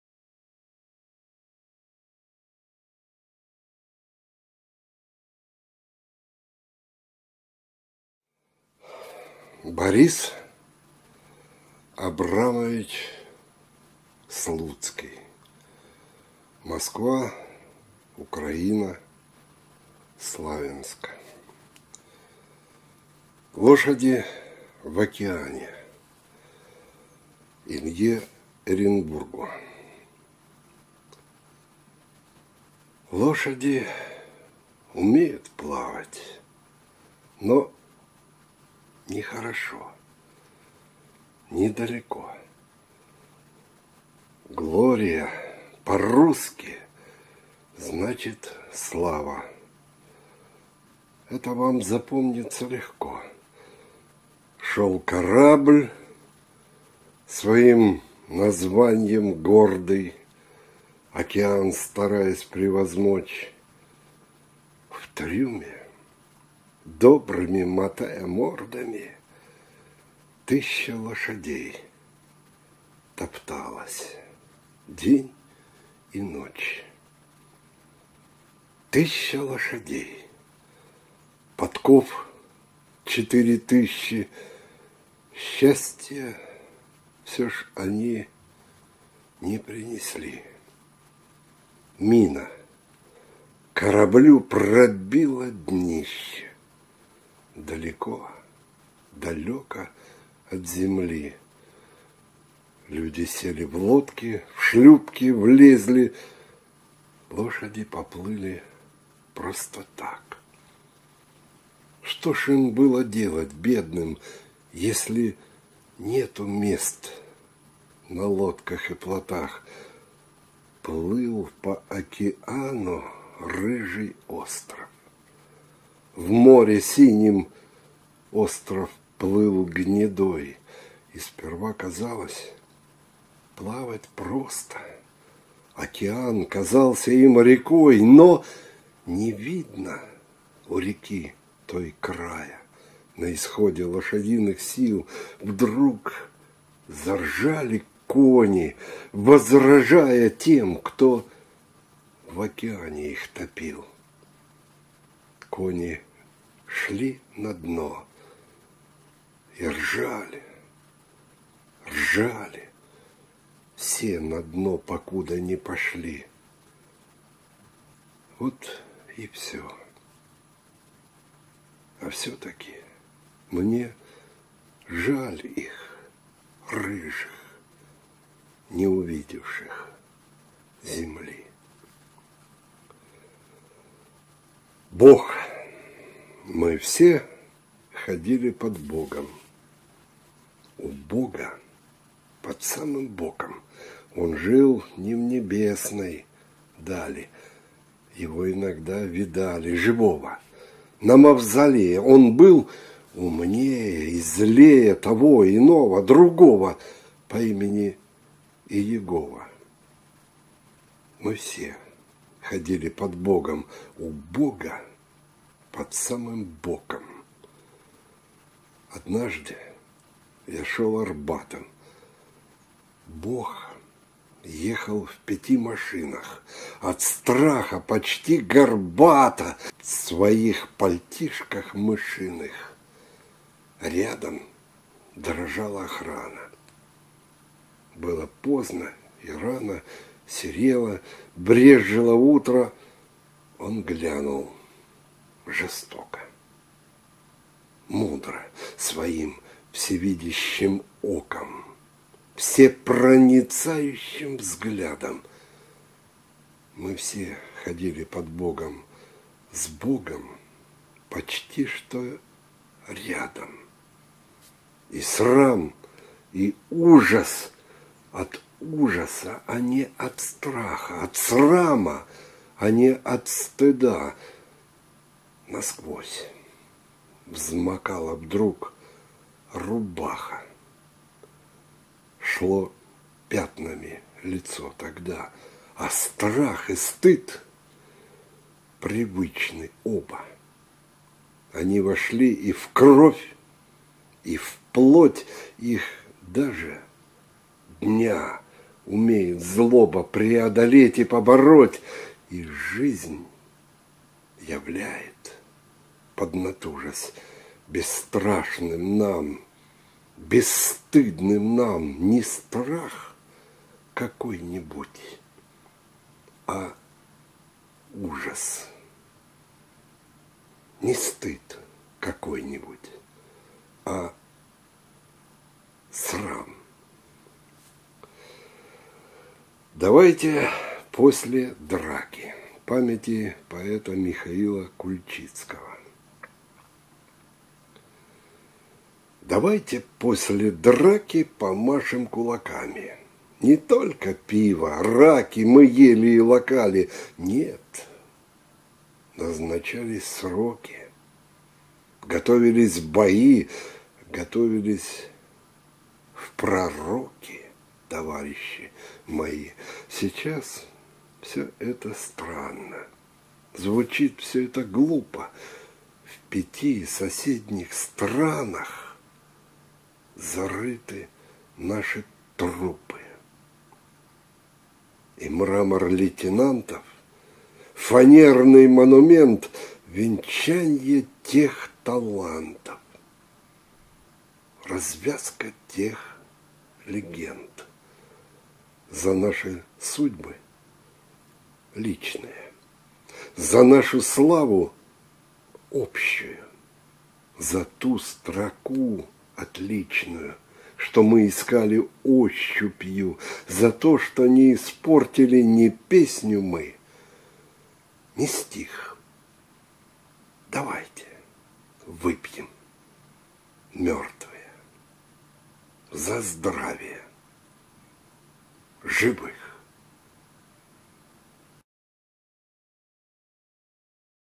Слуцкий Борис Абрамович, 1919-1986 гг. Русские поэты. XX век
Лошади в океане
И.Эренбургу

Лошади умеют плавать,
Но — не хорошо. Недалеко.

«Глория» — по-русски — значит «Слава», -
Это вам запомнится легко.

Шёл корабль, своим названьем гордый,
Океан стараясь превозмочь.

В трюме, добрыми мотая мордами,
Тыща лощадей топталась день и ночь.

Тыща лошадей! Подков четыре тыщи!
Счастья все ж они не принесли.

Мина кораблю пробила днище
Далеко-далёко от земли.

Люди сели в лодки, в шлюпки влезли.
Лошади поплыли просто так.

Что ж им было делать, бедным, если
Нету мест на лодках и плотах?

Плыл по океану рыжий остров.
В море в синем остров плыл гнедой.

И сперва казалось — плавать просто,
Океан казался им рекой.

Но не видно у реки той края,
На исходе лошадиных сил

Вдруг заржали кони, возражая
Тем, кто в океане их топил.

Кони шли на дно и ржали, ржали,
Все на дно покуда не пошли.

Вот и всё. А всё-таки мне жаль их —
Рыжих, не увидевших земли.

Бог

Мы все ходили под богом.
У бога под самым боком.
Он жил не в небесной дали,
Его иногда видали
Живого. На Мавзолее.
Он был умнее и злее
Того — иного, другого,
По имени Иегова…
Однажды я шел Арбатом,
Бог ехал в пяти машинах.
От страха почти горбата
В своих пальтишках мышиных
Рядом дрожала охрана.
Было поздно и рано.
Серело. Брезжило утро.
Он глянул жестоко, — мудро
Своим всевидящим оком,
Всепроницающим взглядом.

С богом почти что рядом.
И срам, и ужас
От ужаса, а не от страха,
от срама, а не от стыда
насквозь взмокала вдруг рубаха,
шло пятнами лицо тогда.
А страх и стыд привычны оба.
Они вошли и в кровь, и в плоть.
Их даже
дня
умеет
злоба
преодолеть и побороть.
И жизнь являет, поднатужась,
бесстрашным нам,
бесстыдным нам
не страх какой-нибудь, а ужас,
не стыд какой-нибудь, а срам.

Давайте, после драки…

Памяти поэта Михаила Кульчицкого

Давайте после драки
Помашем кулаками,
Не только пиво-раки
Мы ели и лакали,
Нет, назначались сроки,
Готовились бои,
Готовились в пророки
Товарищи мои.

Сейчас все это странно,
Звучит все это глупо.
В пяти соседних странах
Зарыты наши трупы.
И мрамор лейтенантов —
Фанерный монумент —
Венчанье тех талантов,
Развязка тех легенд.

За наши судьбы (личные),
За нашу славу (общую),
За ту строку отличную,
Что мы искали ощупью,
За то, что не испортили
Ни песню мы, ни стих,
Давайте выпьем, мертвые,
За здравие живых!